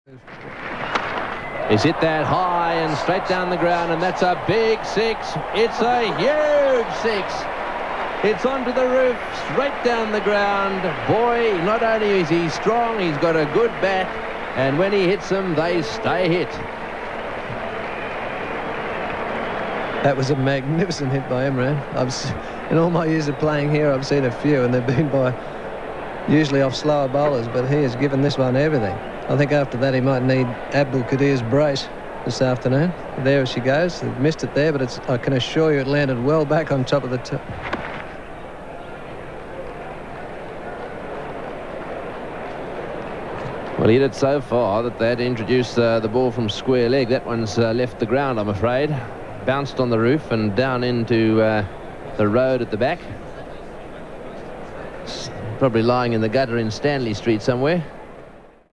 MASSIVE! Imran Khan puts Terry Alderman onto the roof at the Gabba ODI January 1989
What a player! The Alpha male. Imran puts Alderman into the next post code with this massive straight hit at the Gabba in January 1989. @fromashestoarchive1861